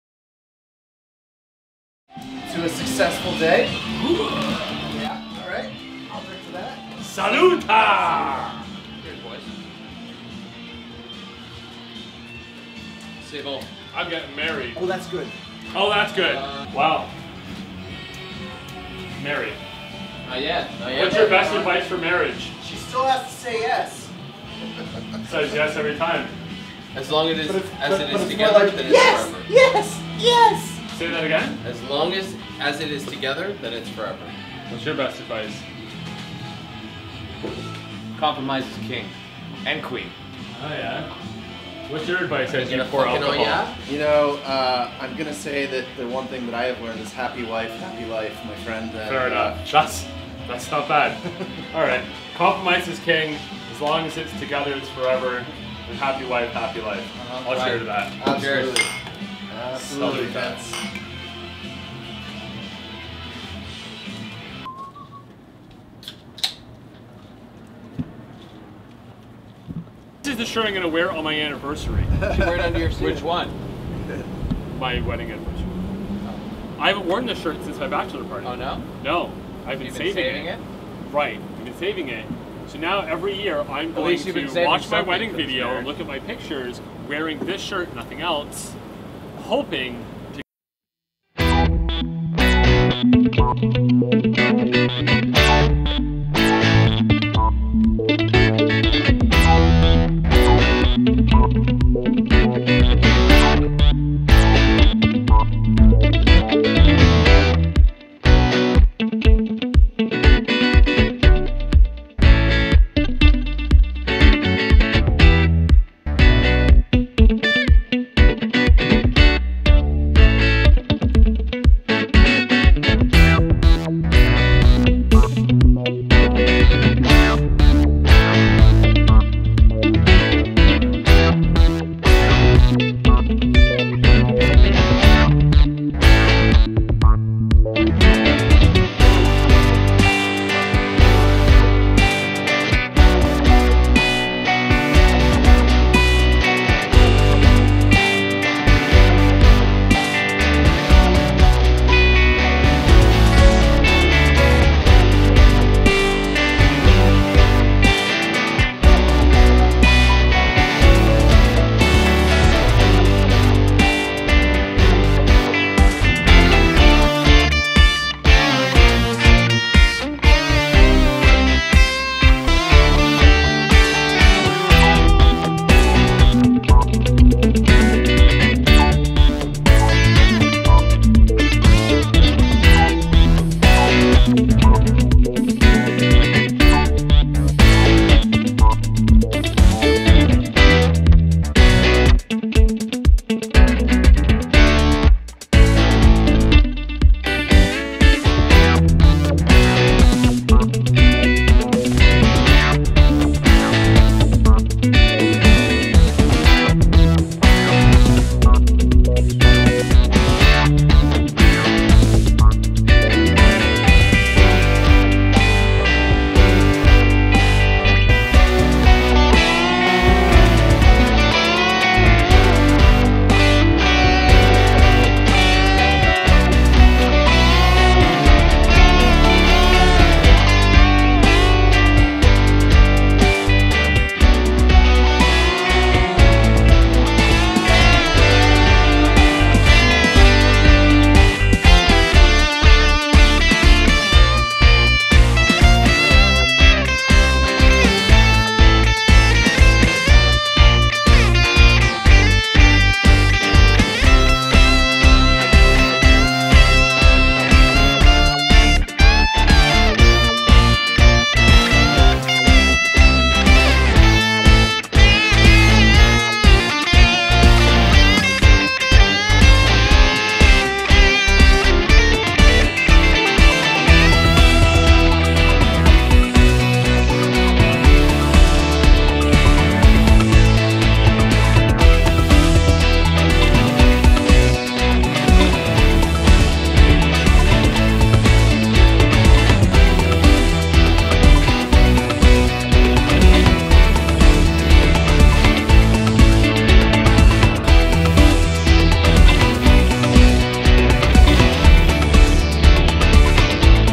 Groom preparation. Kat and Chris wedding.
Beautiful wedding of Kat & Chris. Groom preparation